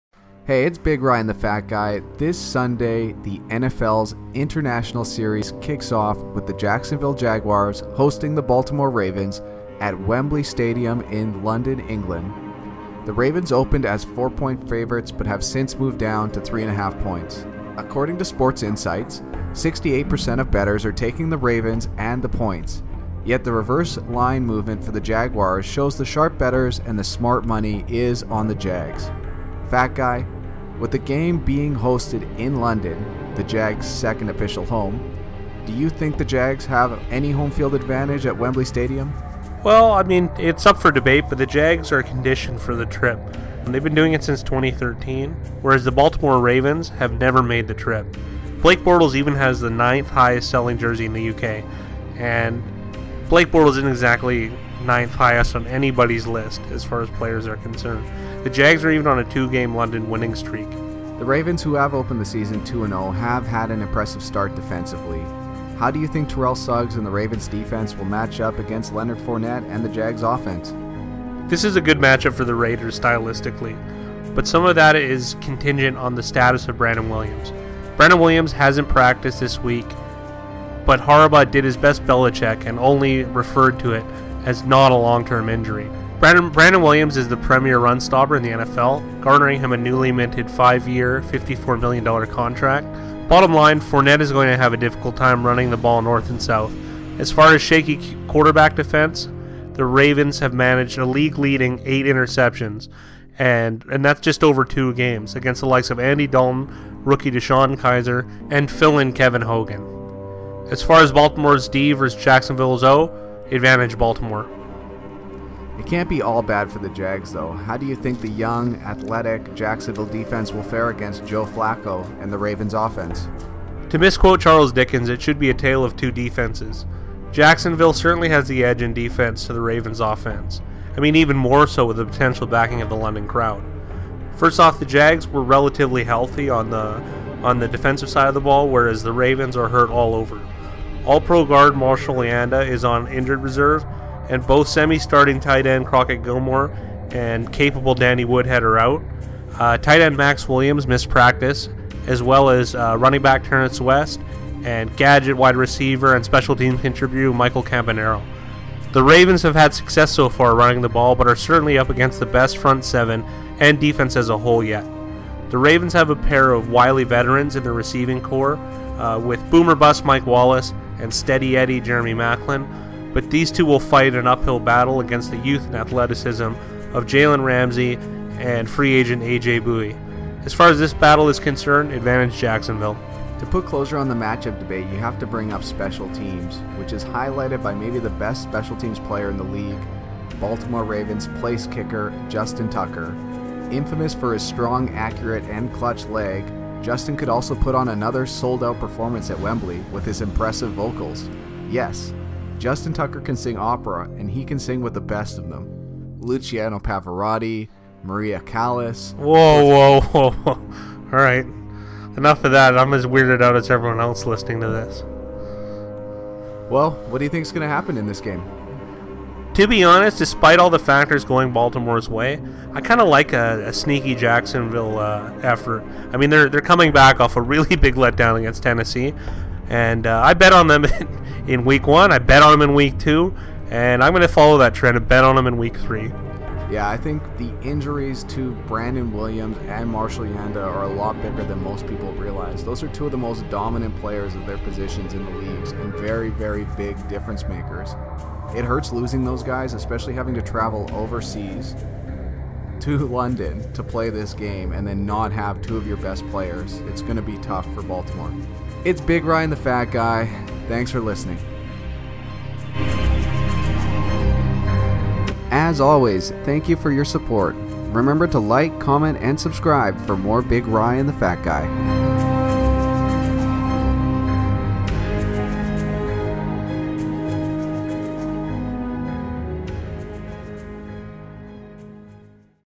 Ravens vs Jaguars | Week 3 Preview | NFL International Series
The Jacksonville Jaguars host the Baltimore Ravens at Wembley Stadium in London England. How will Joe Flacco cope with the loss of All-Pro Guard Marshall Yanda while facing the young and ferocious Jags Defensive Line? Will Jacksonville be able to establish an edge at the line of scrimmage on Offense to allow Leonard Fournette to punish defenders at the second level?